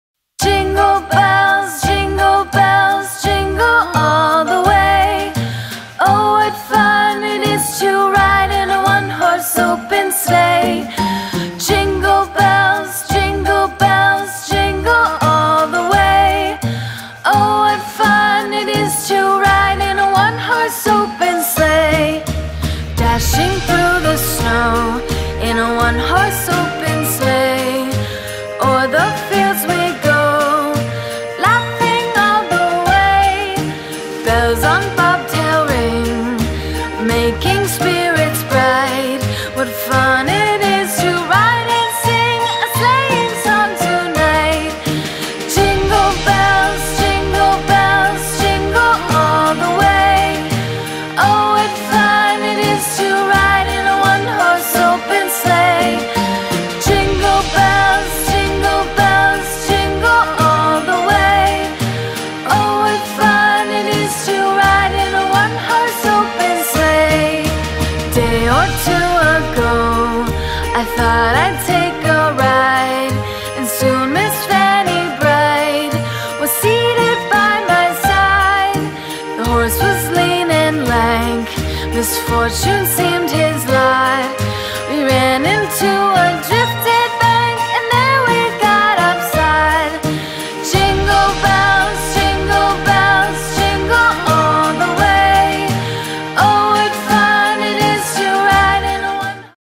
DIY - Decoratiuni de iarnă, din stelute de hartie
Dacă aveți timp, poate vă inpiră decorațiunile noastre de iarnă, din steluțe de hârtie!